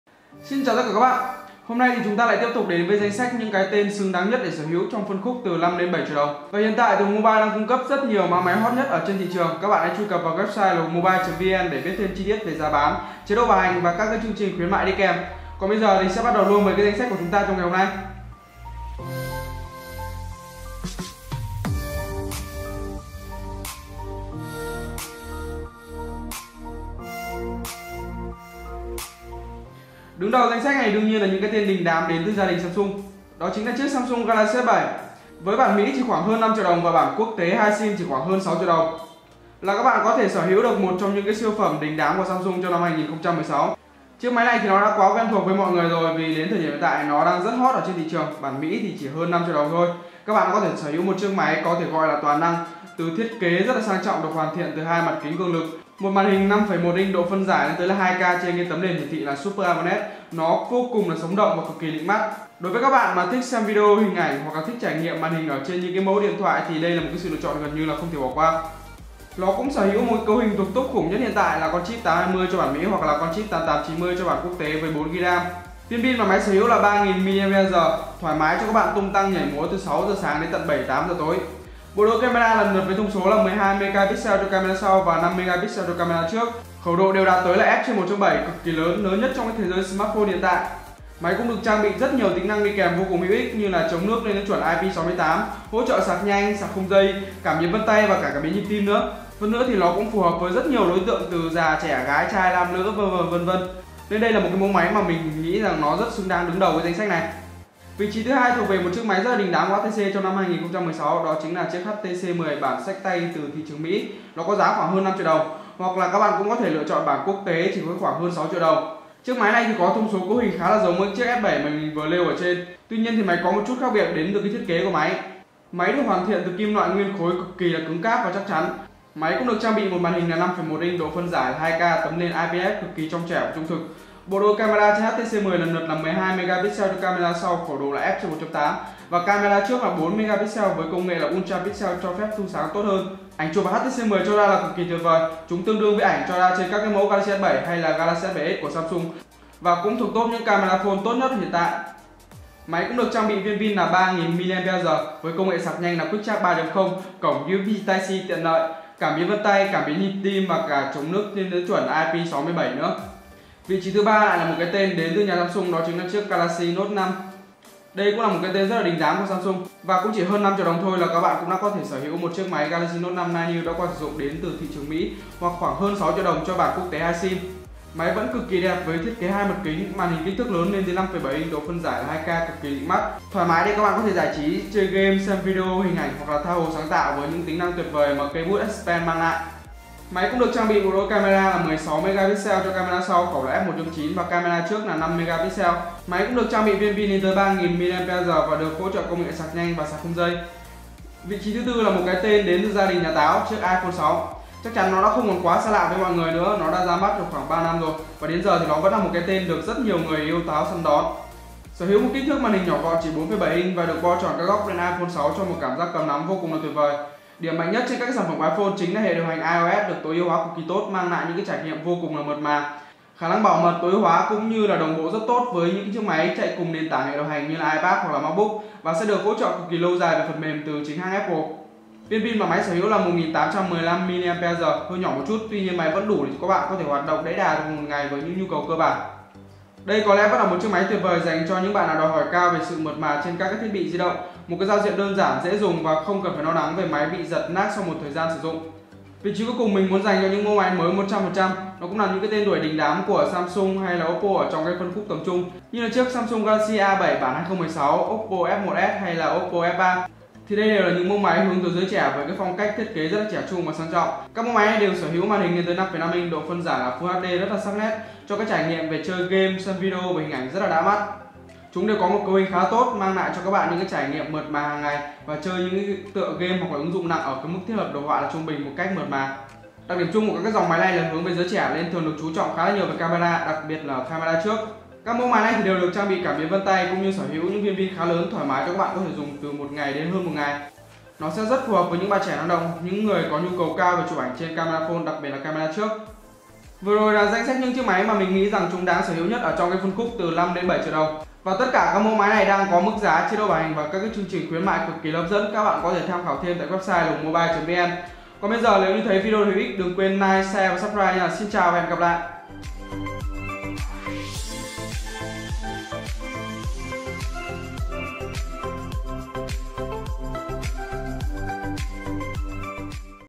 Top điện thoại tầm giá 5 đến 7 triệu đồng nên mua nhất hè 2017 | HungMobile
5 đến 7 triệu là khoảng giá rất phù hợp với nhiều người để mua sắm điện thoại. Thị trường công nghệ luôn luôn biến động rất nhanh với nhiều sản phẩm được ra mắt liên tục, rất khó để cho bạn có thể bắt kịp được để đưa ra những lựa chọn tốt nhất cho mình. Và chắc hẳn, bạn cũng là người đã phân vân, không biết lựa chọn nào sẽ phù hợp nhất và đáp ứng được đầy đủ nhu cầu của bạn.
Sau đây, HungMobile sẽ đưa ra những sự lựa chọn xứng đáng nhất giành cho bạn. Trong đó sẽ có cả sự lựa chọn đối với hàng mới nguyên hộp và cả sự lựa chọn đối với hàng qua sử dụng, để công bằng nhất về sở thích của mọi người. Hi vọng các bạn sẽ có những gợi ý hữu ích.

- ADD: Số 61 ngõ 16 - Thái Hà - Hà Nội.

Cảm ơn các bạn đã theo dõi video. Hãy để lại ý kiến bình luận bên dưới để cùng nhau tham khảo. Xin chào và hẹn gặp lại ở các video tiếp theo.